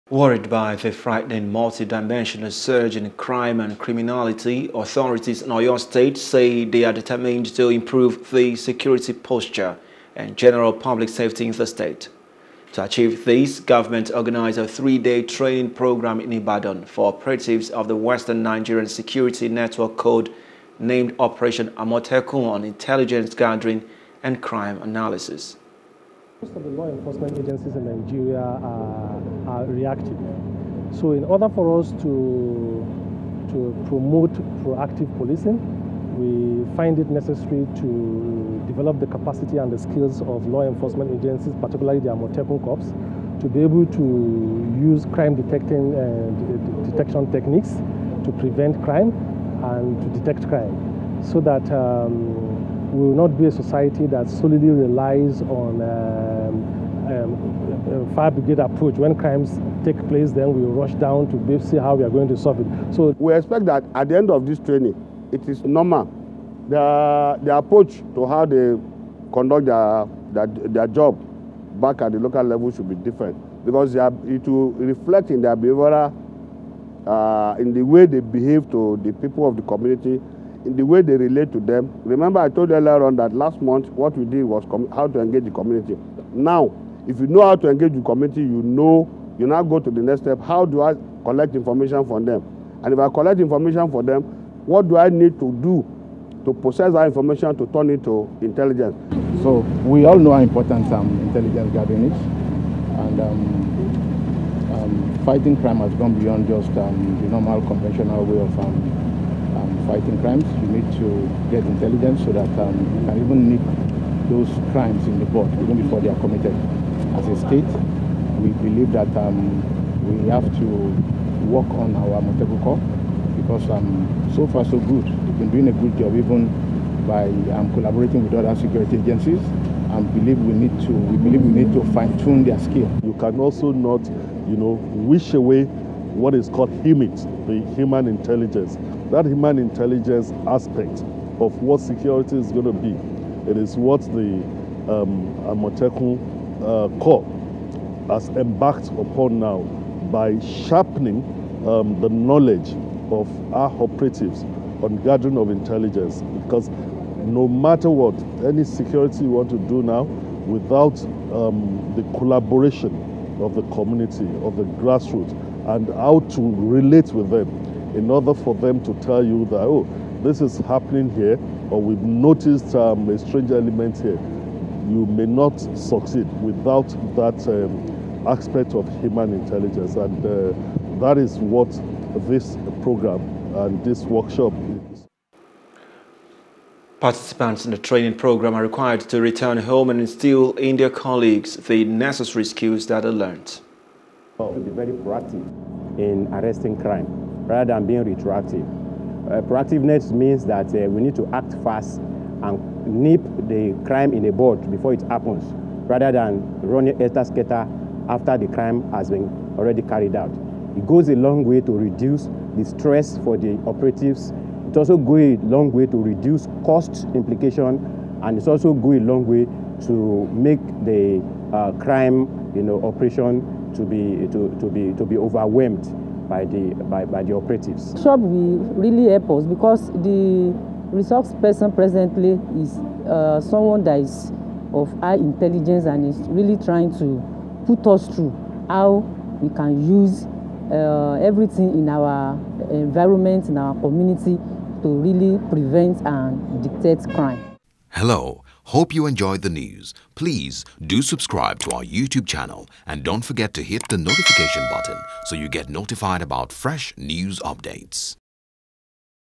Oyo Govt Trains Amotekun Corps On Local Intelligence, Reiterates Commitment To Improve Security
Worried by the frightening multi-dimensional surge in crime and criminality, authorities in Oyo state say they are determined to improve the security posture and general public safety in the state.